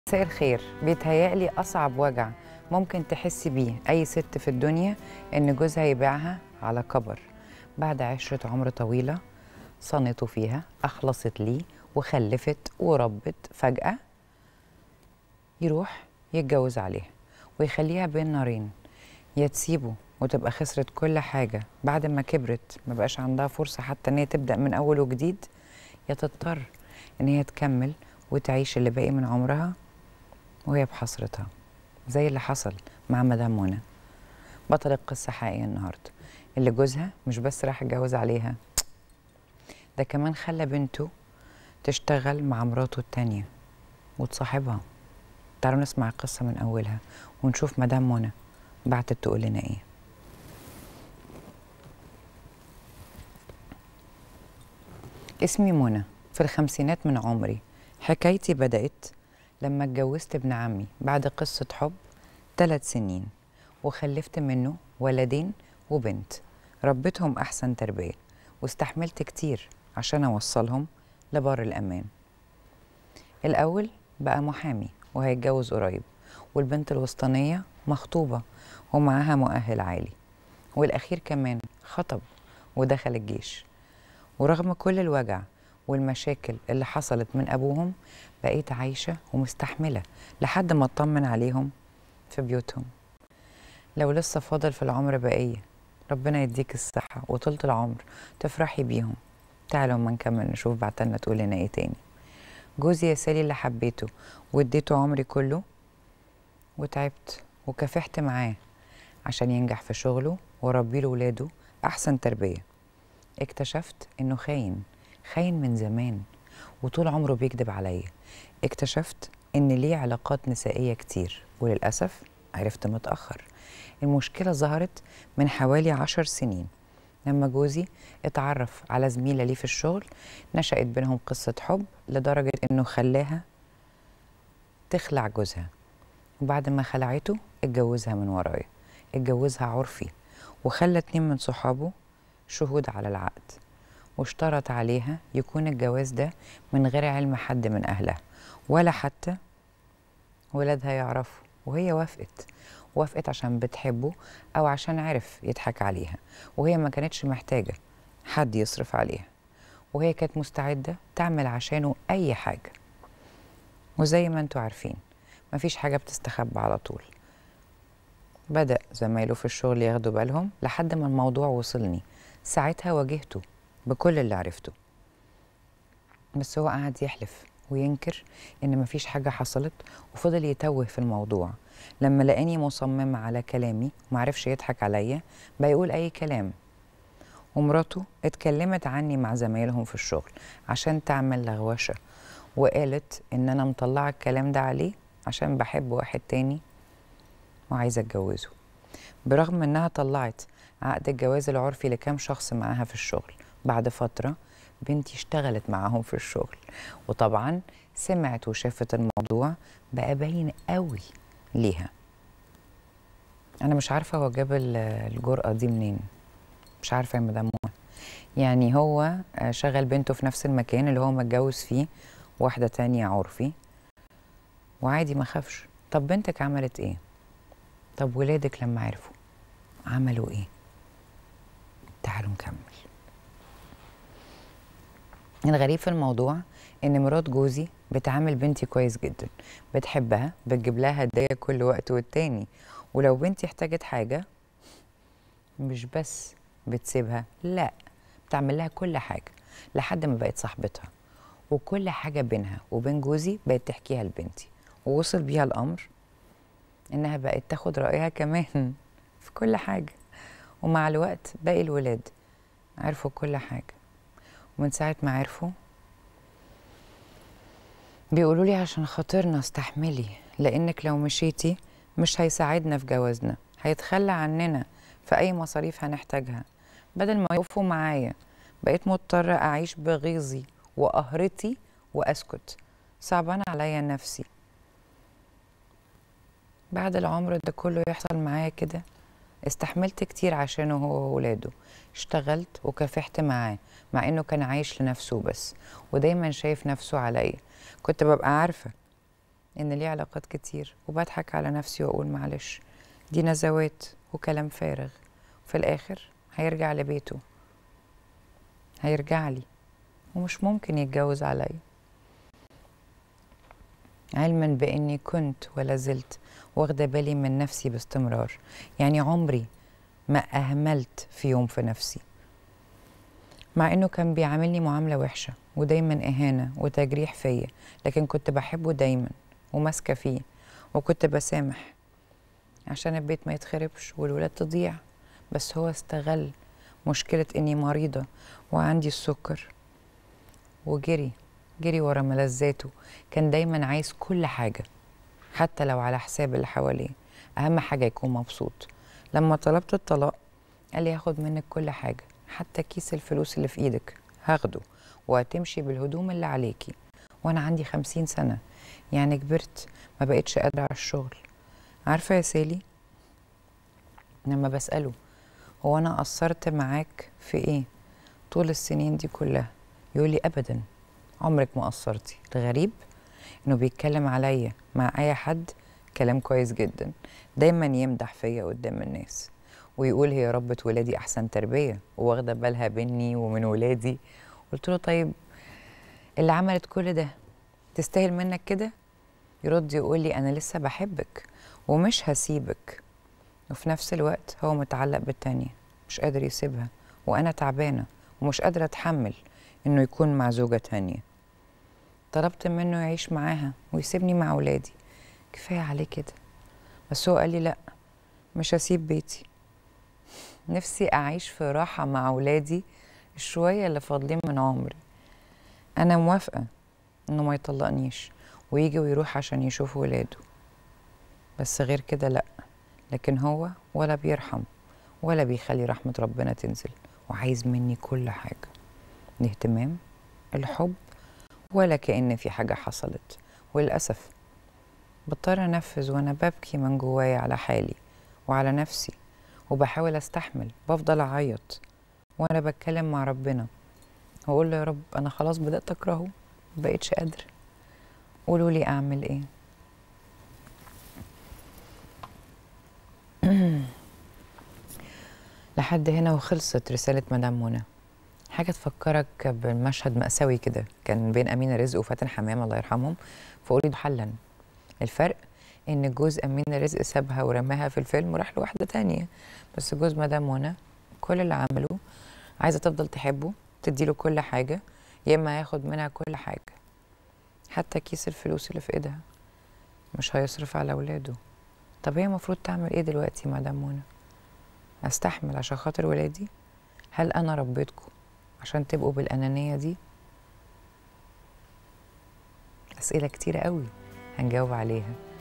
“بعد عشرة العمر، أكتشفت أنه متجوز عليا عرفي💥“. حكاية مختلفة جدا اعرف قصة “منى“ من سالي عبد السلام 👇🏻
“ بعد عشرة العمر، أكتشفت أنه متجوز عليا عرفي وخلى زميلته في الشغل تخلع جوزها💥💥“ .. حكاية مختلفة جدا اعرف قصة “ منى “ من سالي عبد السلام 👇🏻